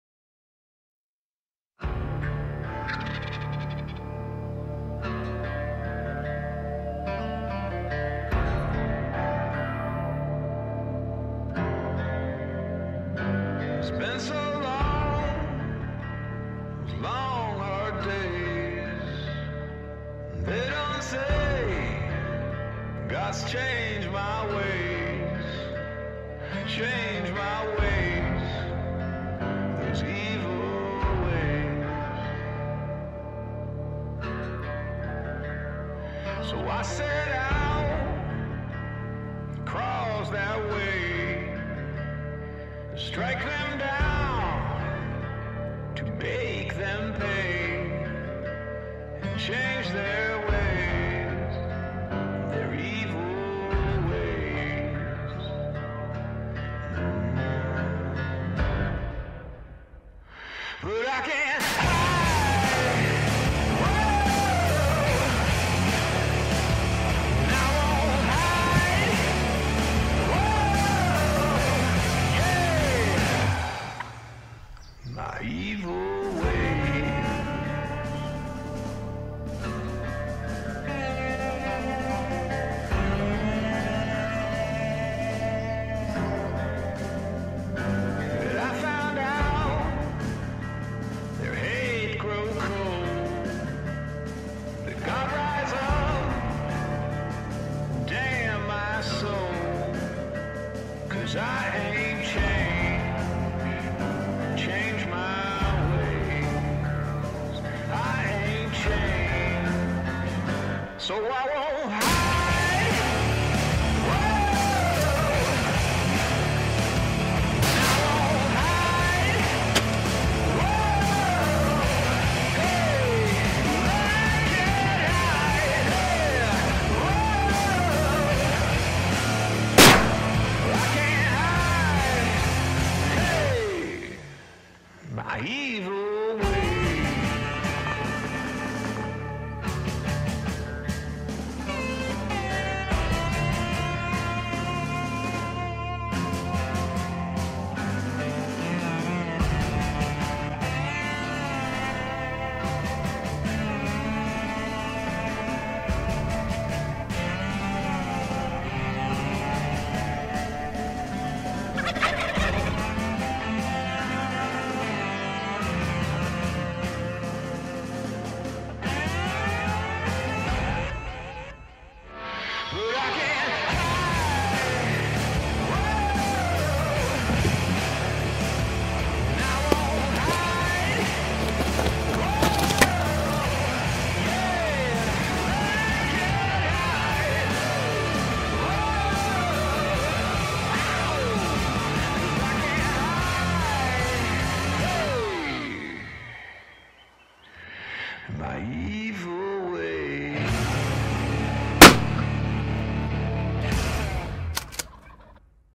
Dave Smith Decoys Video Entry
This is my entry into the DSD video contest. These are hands down the best decoys on the market!!! If you have not hunted over any you do not know what you are missing!!!